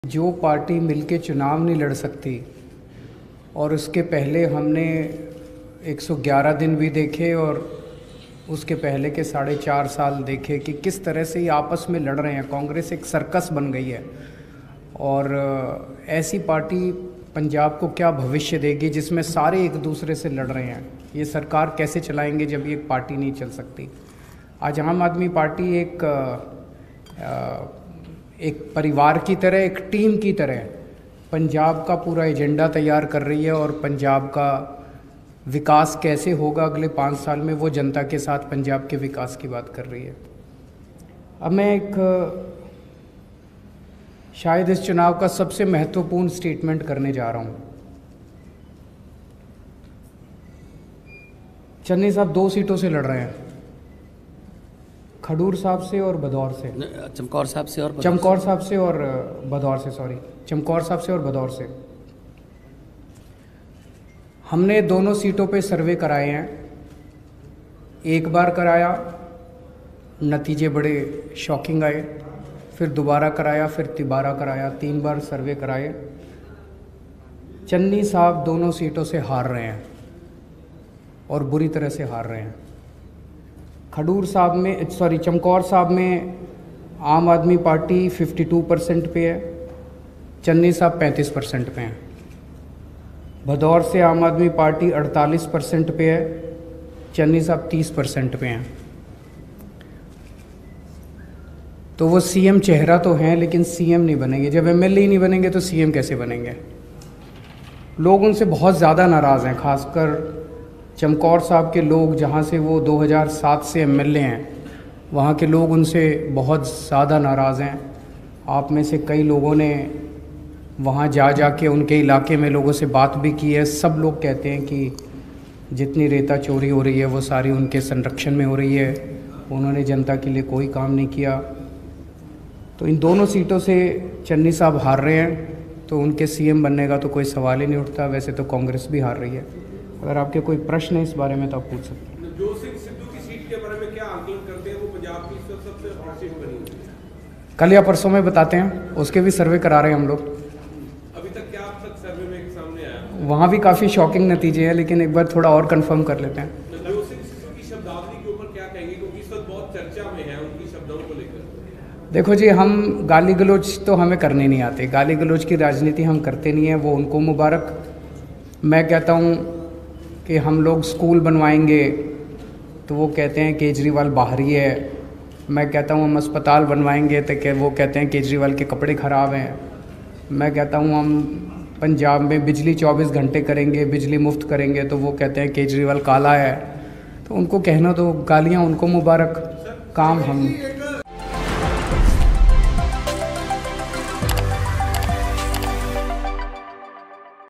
Punjab Election 2022: Arvind Kejriwal का Congress CM Channi पर अटैक | Bhagwant Mann AAP
पंजाब विधानसभा चुनाव से पहले आरोप-प्रत्यारोप का दौर तेज हो गया है। अमृतसर में AAP के राष्ट्रीय संयोजक अरविंद केजरीवाल ने कांग्रेस पार्टी और चरणजीत सिंह चन्नी पर तीखा हमला बोला। केजरीवाल ने कहा कि चन्नी साहब 2 सीटों से चुनाव लड़ रहे हैं, हमने दोनों सीटों पर तीन बार सर्वे कराया और वो दोनों सीटों पर हार रहे हैं। चमकौर साहिब में AAP 52% और चन्नी साहब 35% पर हैं. भदौर से AAP 48% और चन्नी साहब 30% पर हैं. जब वे MLA ही नहीं बनेंगे तो सीएम क्या बनेंगे?